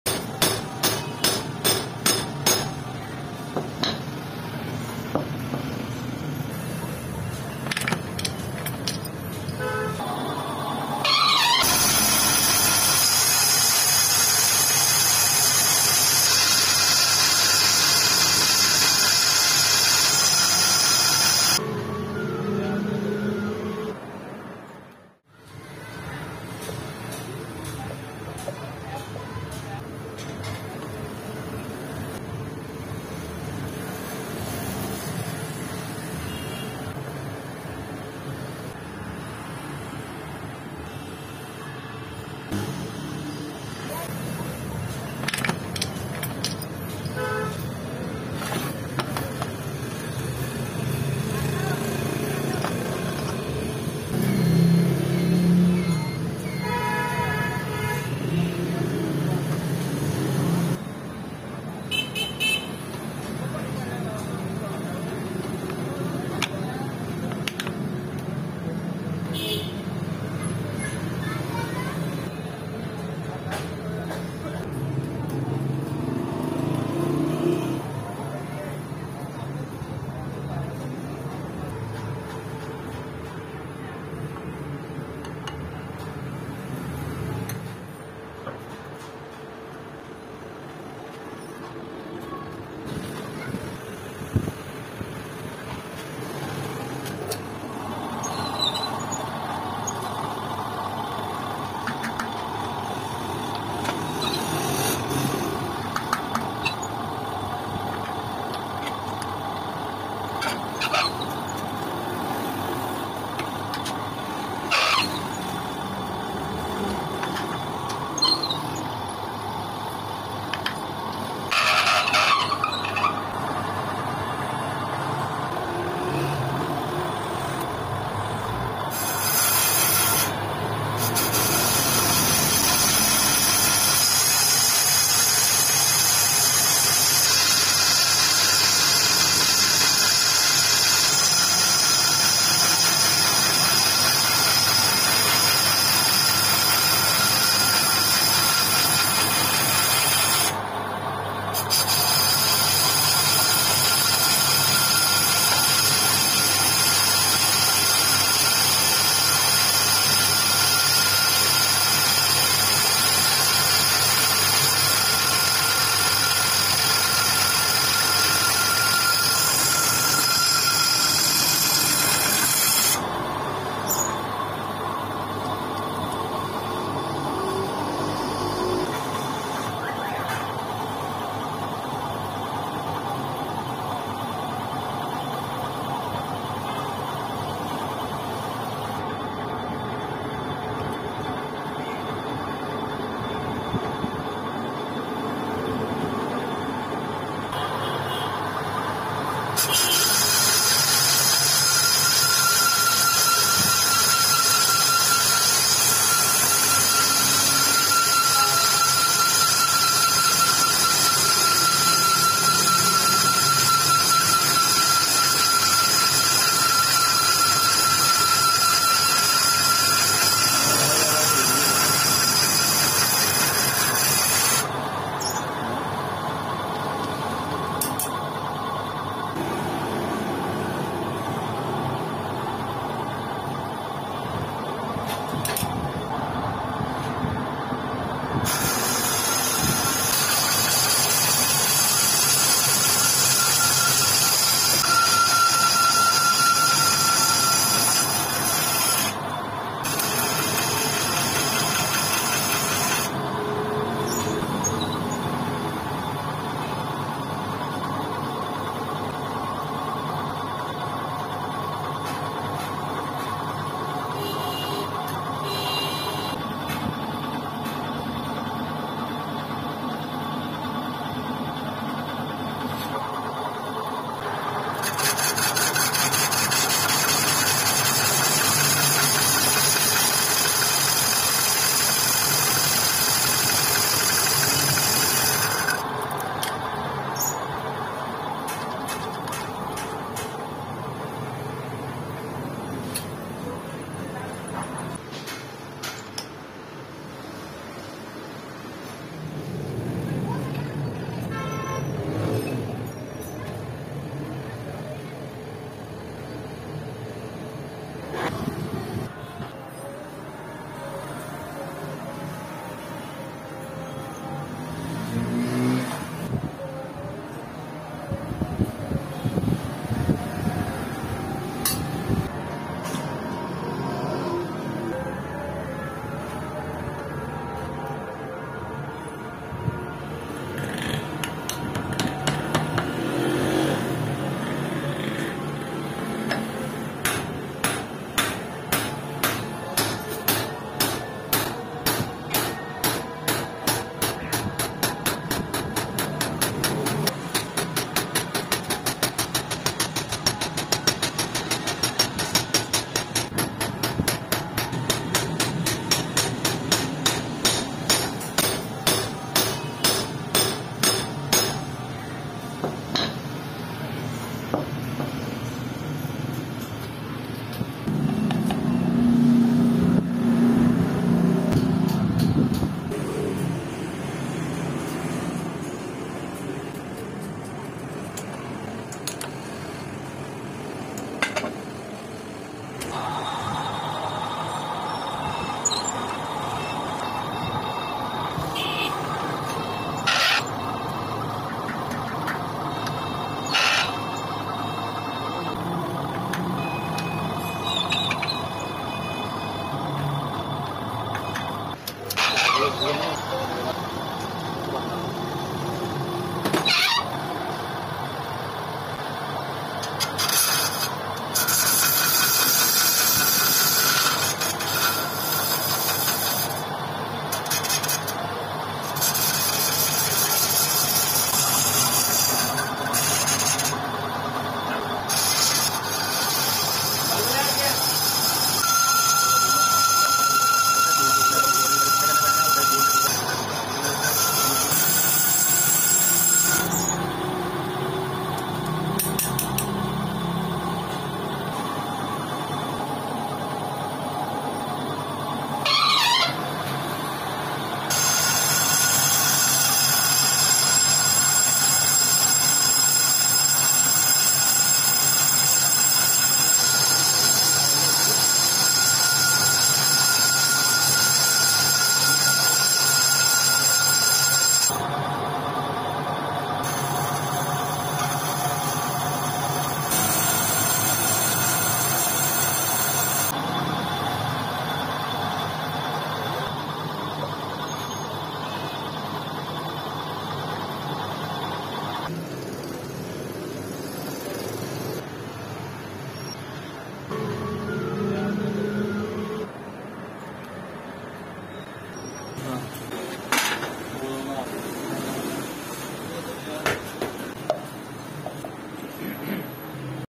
How to repair and resurface motorbike brake - Zimbiker
Few People Know, how to repair motorbike hub wheel brake.
Motorcycle Wheel Hub Brake resurfacing and repairing on lathe machine.